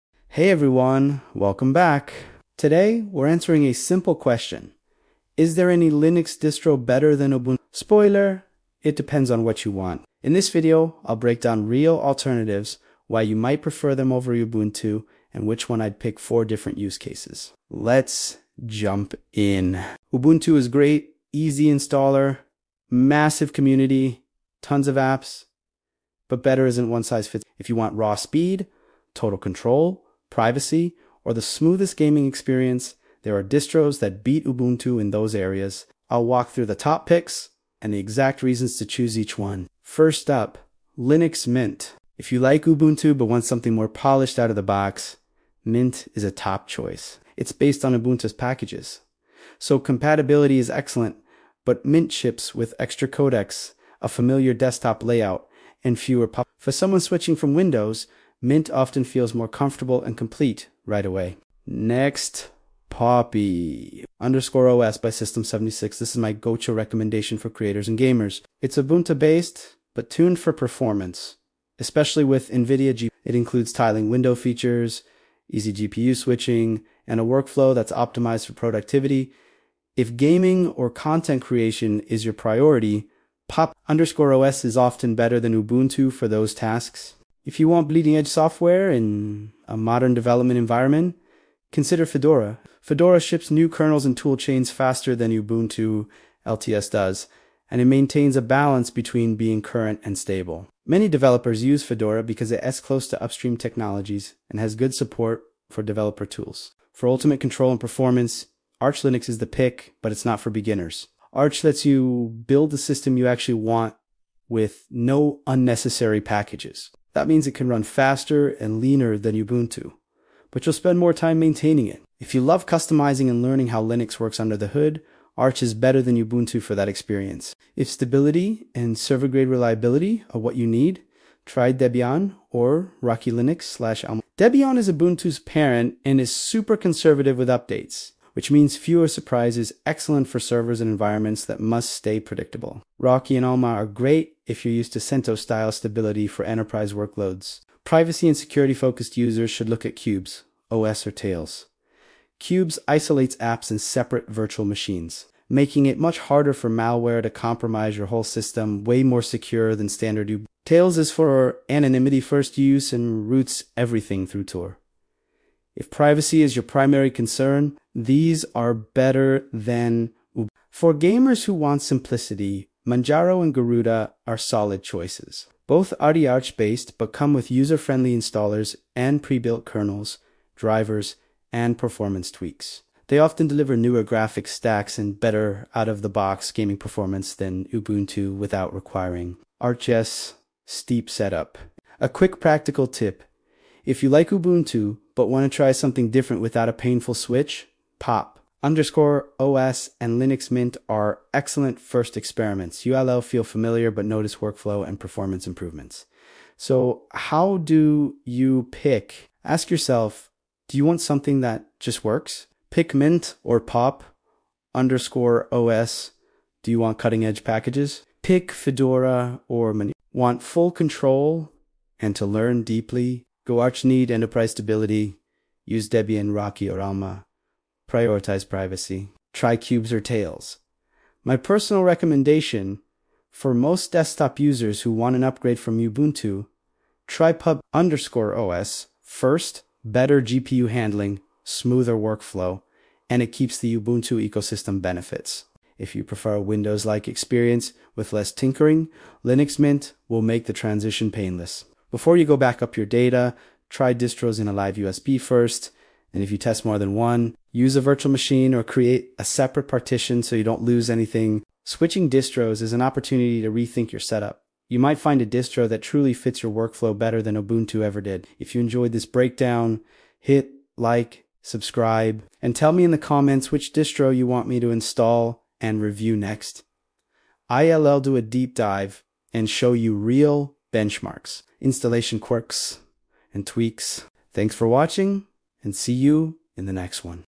Better Than Ubuntu? Top Linux Distros You Should Be Using in 2025!
Is Ubuntu really the best Linux distro in 2025? 🤔 In this video, we explore the top Linux alternatives that outperform Ubuntu in speed, stability, gaming, and privacy. From Pop!_OS to Linux Mint, Fedora, Arch, and more — discover which distro fits your needs best! Whether you’re a beginner, gamer, or developer, there’s a perfect Linux for you.

💻 Watch till the end to find out my personal recommendation for the best Ubuntu alternative!